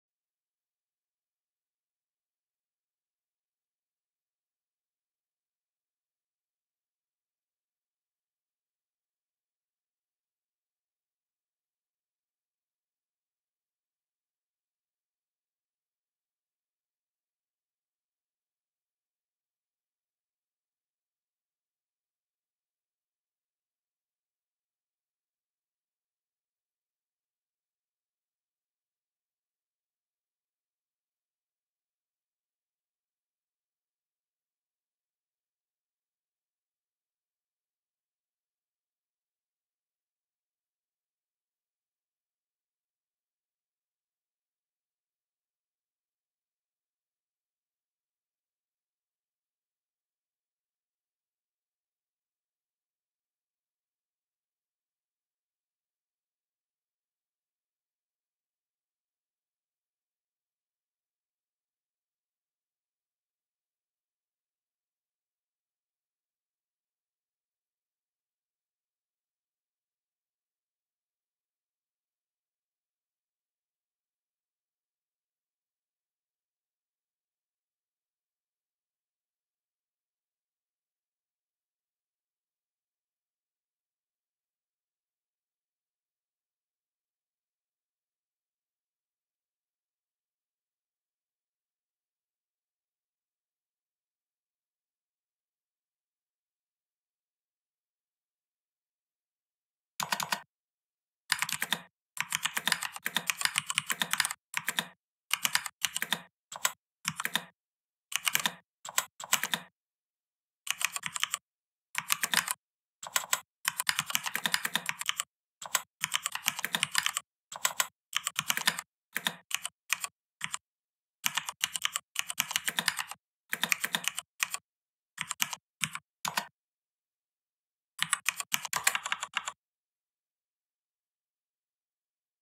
how to make ure keyboard sound like a typewriter as
yooooo today ima teach yall how to make ure keyboard sound like an old fashioned typewriter.